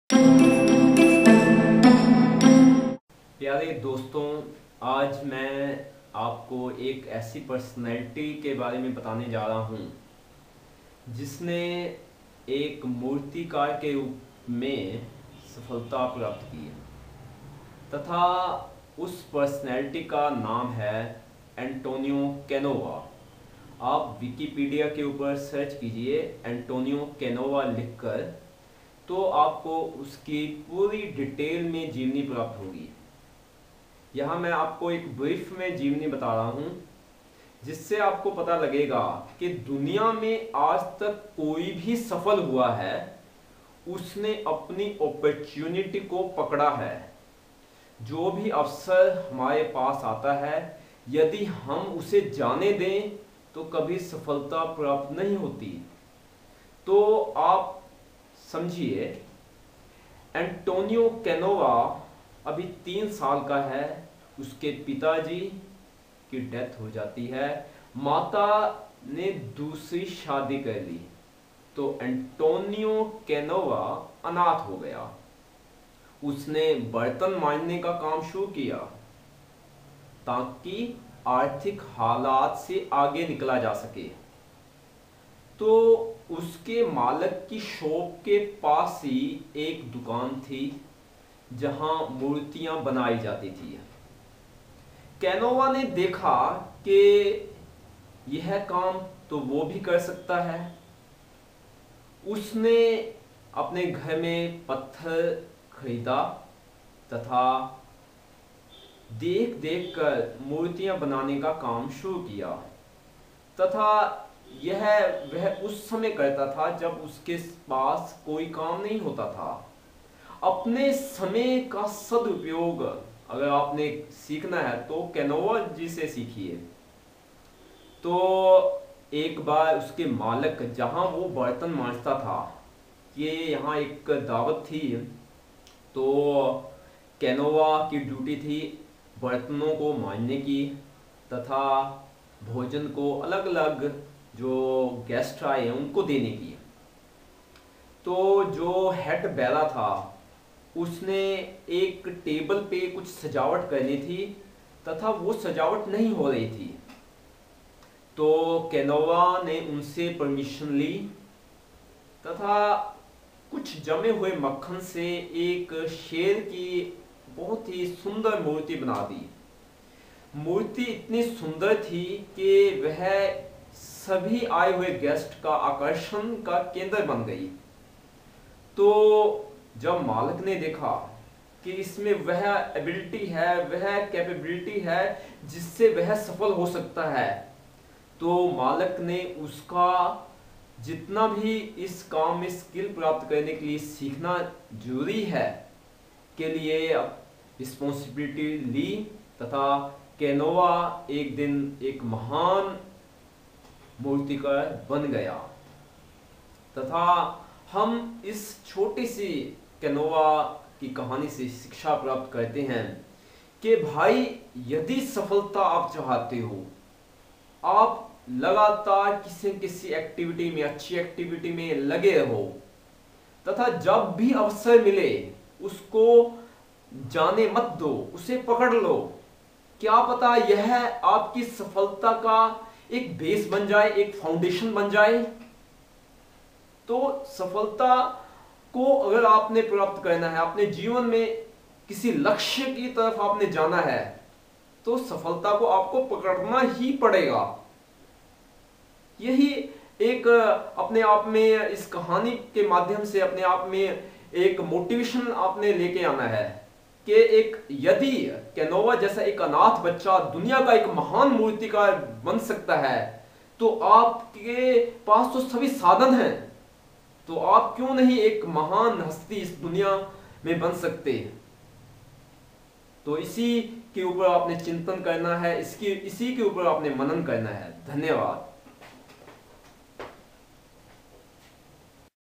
Antonio Canova- Inspiration and Motivation
Biography of Antonio Canova is most inspirational and motivational story for me. His father died and his mother remarried when he was 3 years old. He was orphan but he was great passion to become successful in the life. He started to work as cleaner of utensils in a hotel. But in his free time, he learned the art of sculpture. He did not waste his time. He was also expert to capture his opportunity.

See More Inspirational and Motivational Videos at